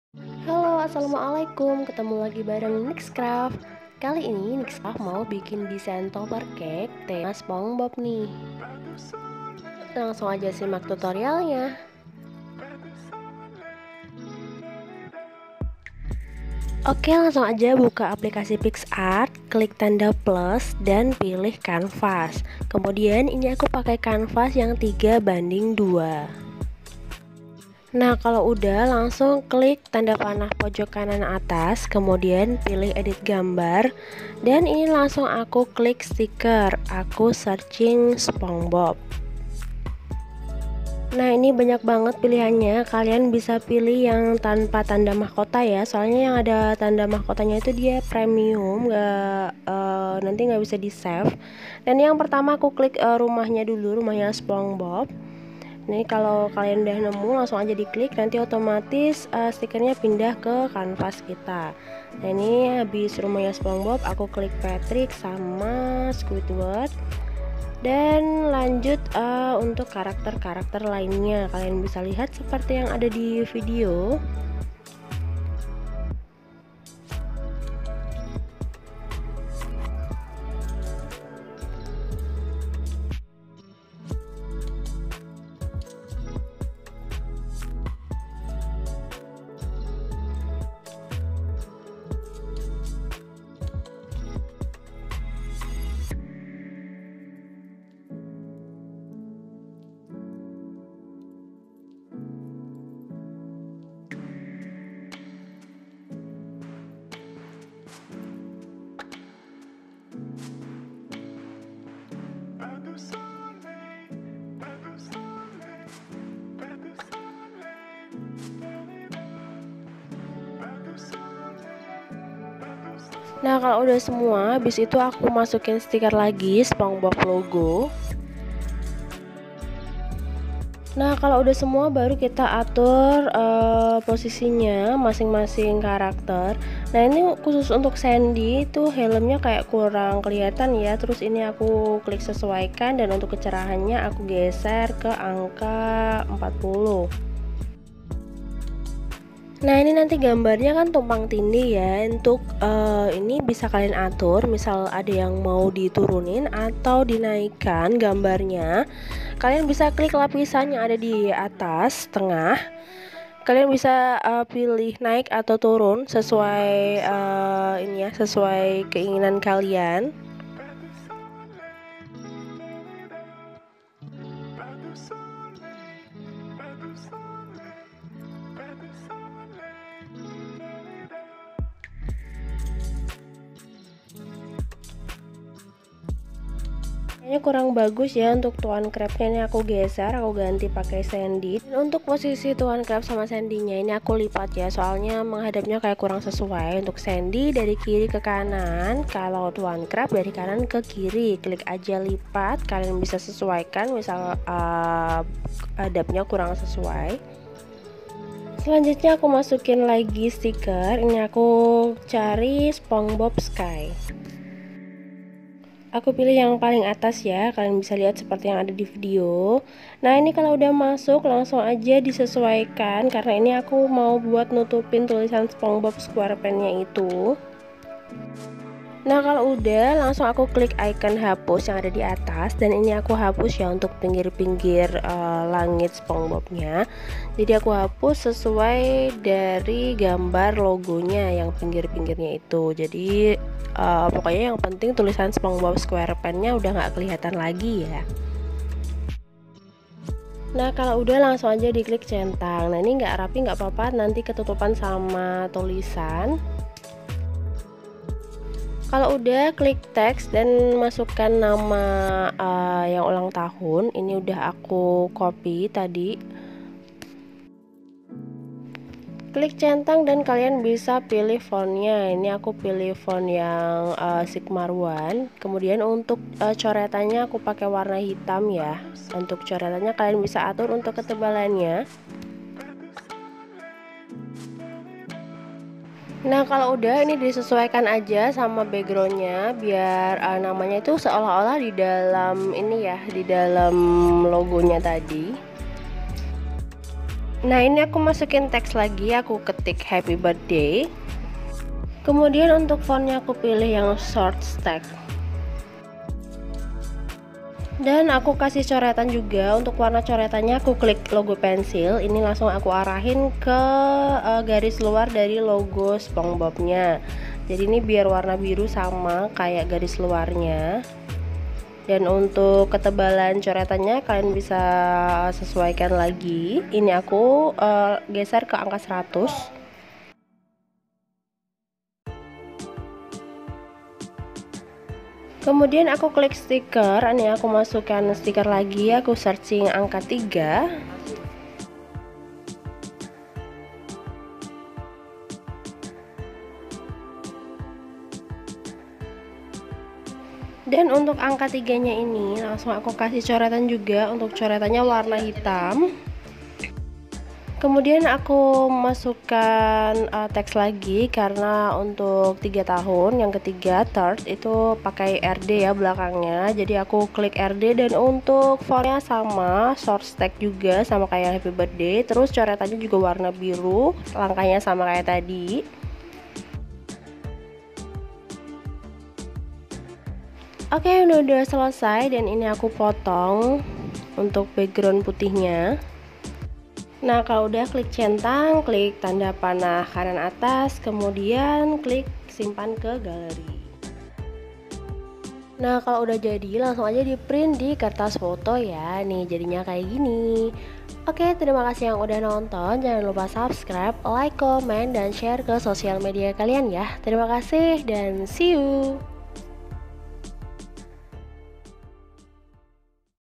DIY DESAIN TOPPER CAKE SPONGEBOB SQUAREPANTS || MEMBUAT TOPPER CAKE PAKAI APLIKASI PICSART
Hai, Assalamualaikum.. Di video ini aku share ke kalian CARA MEMBUAT DESAIN TOPPER CAKE, kali ini tema SpongeBob SquarePants yaa..

Caranya gampang banget deh!!! CUKUP 1 APLIKASI yaitu PICSART!!!

Kalian juga bisa tambahkan foto anak ya, caranya juga sama..
kalau desain udah jadi, bisa di print di kertas foto atau kertas HVS terus dilaminating..

Yuukk langsung simak tutorialnya!!
Selamat mencoba!!!

Semoga video ini bermanfaat ya buat kita semua..